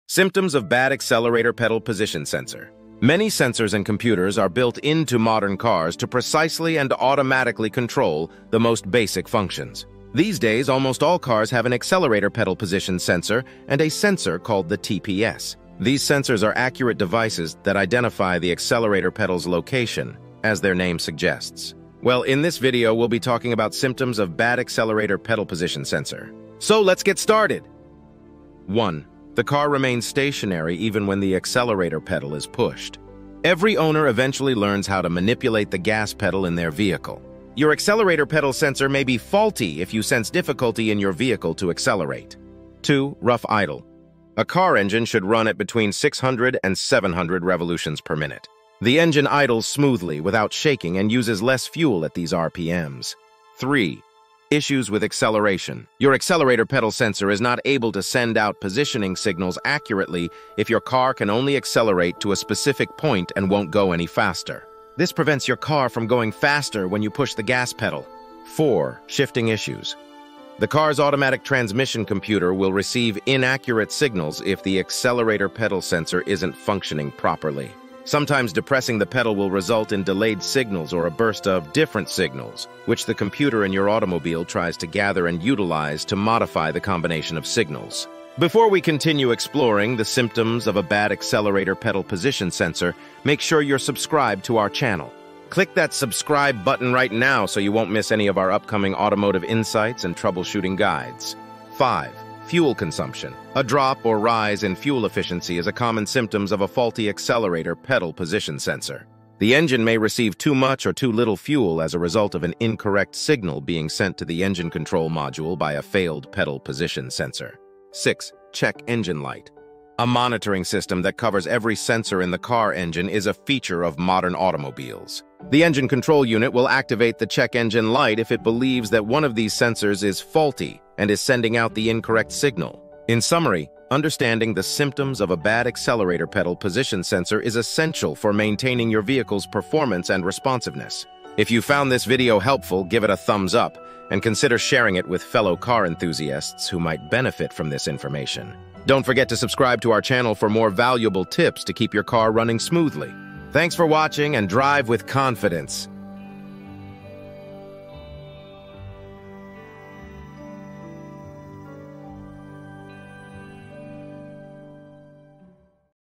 Symptoms of Bad Accelerator Pedal Position Sensor
Welcome to our latest video where we unravel the essential topic of "Symptoms of a Bad Accelerator Pedal Position Sensor." This sensor is a crucial part of your vehicle's throttle control system, and when it's not working correctly, it can affect your car's performance. We'll guide you through common indicators of a malfunctioning accelerator pedal position sensor, why it's vital to address the issue promptly, and how to keep your vehicle running smoothly. Whether you're a seasoned car enthusiast or a vigilant driver, this knowledge is valuable. Let's get your car back on the road with confidence!